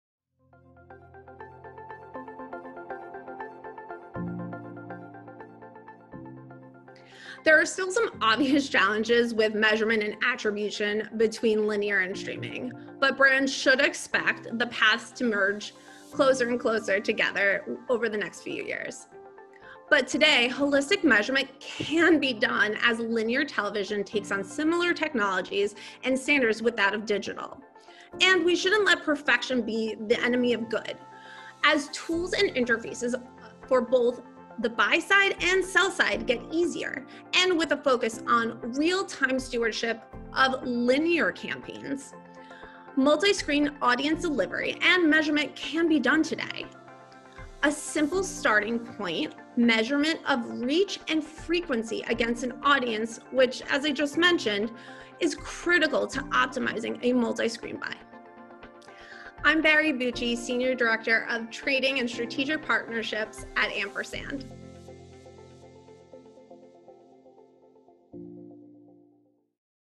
Holistic Video Measurement
In this Magnite Minute, Bari Bucci, Senior Director of Trading & Strategic Partnerships at Ampersand, posits that while there are still challenges to work out with regards to measurement and attribution between linear TV and streaming TV, holistic measurement can be done. A simple starting point is measuring reach and frequency against an audience, which can go a long way in optimizing multi-screen buys.